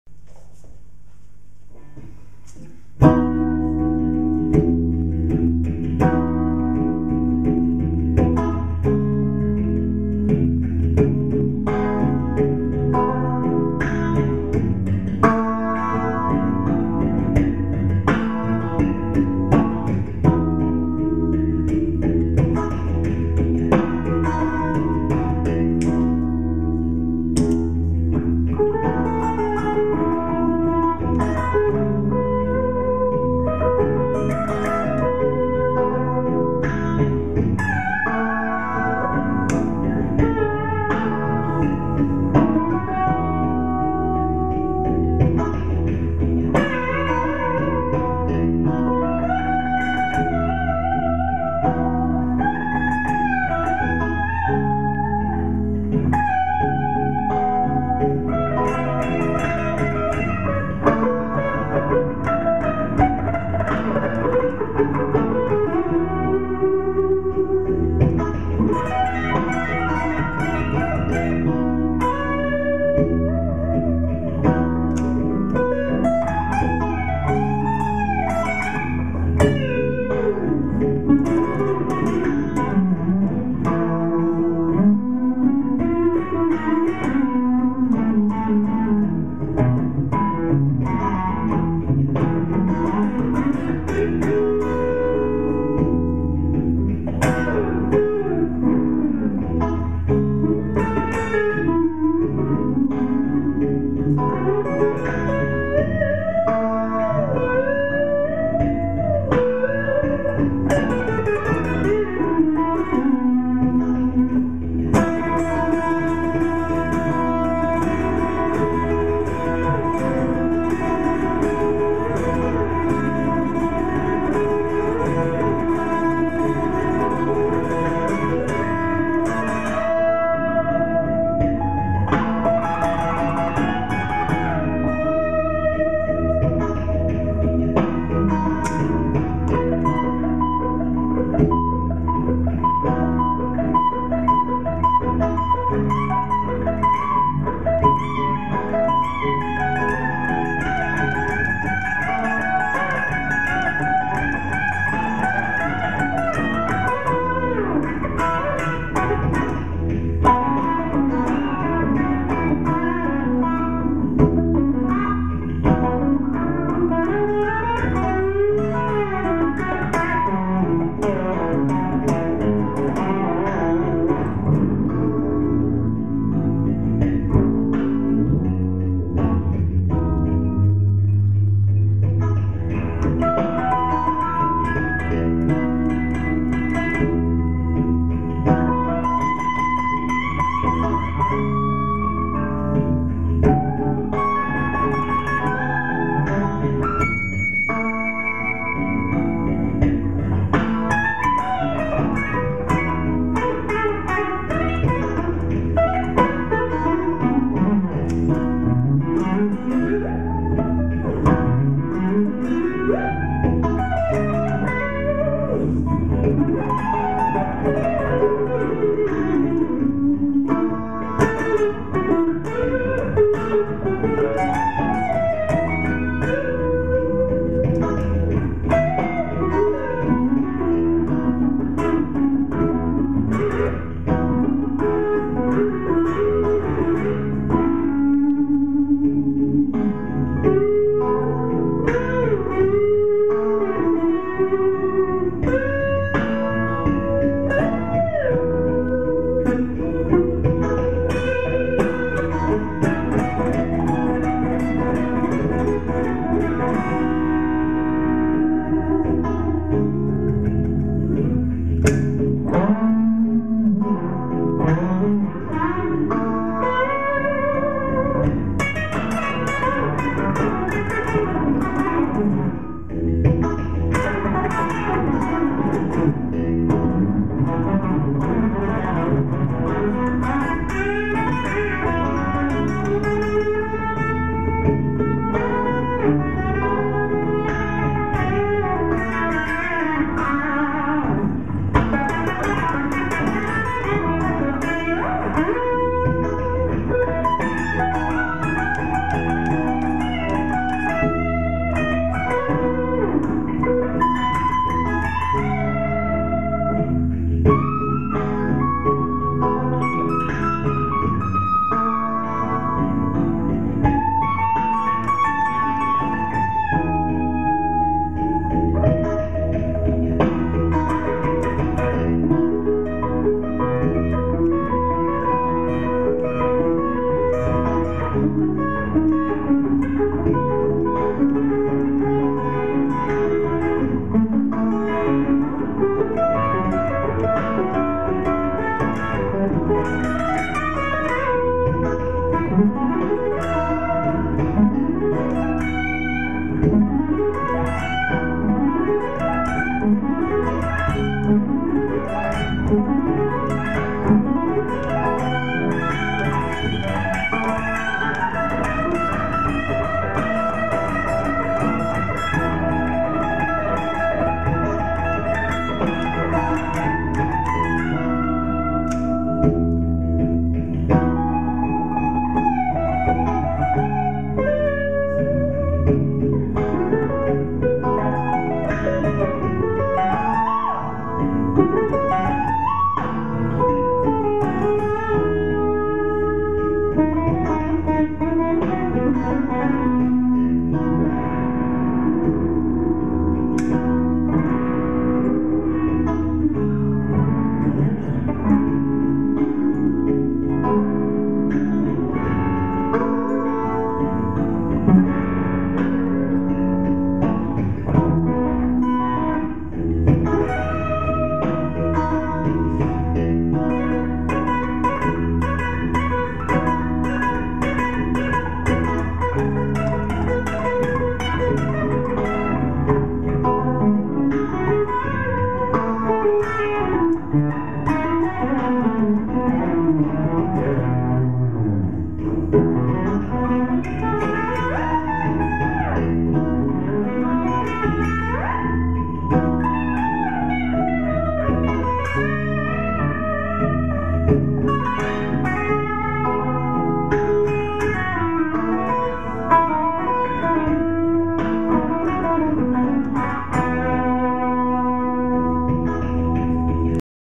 Half drive Hermida Zendrive and Fender amp
half drive Zendrive, Ibanez digital delay, Fender Pro Reverb, no reverb